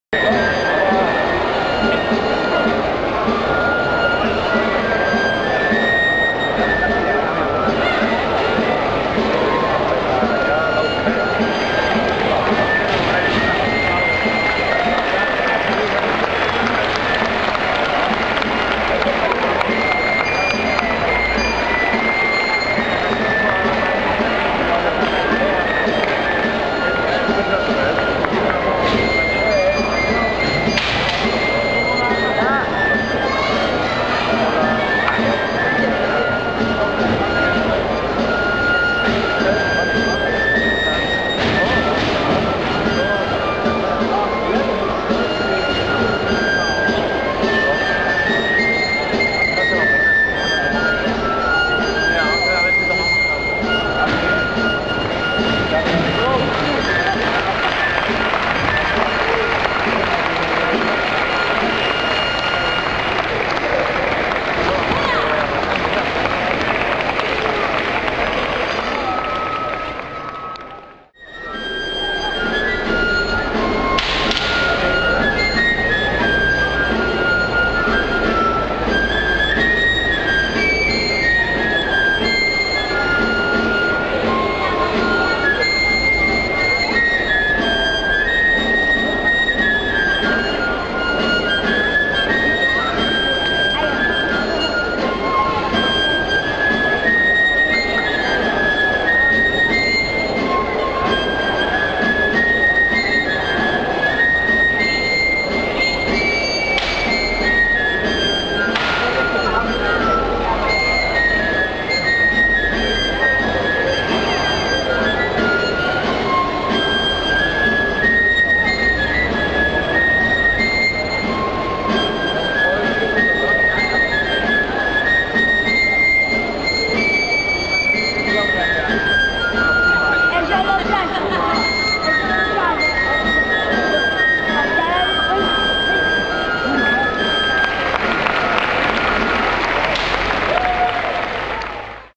Dantzari gazteak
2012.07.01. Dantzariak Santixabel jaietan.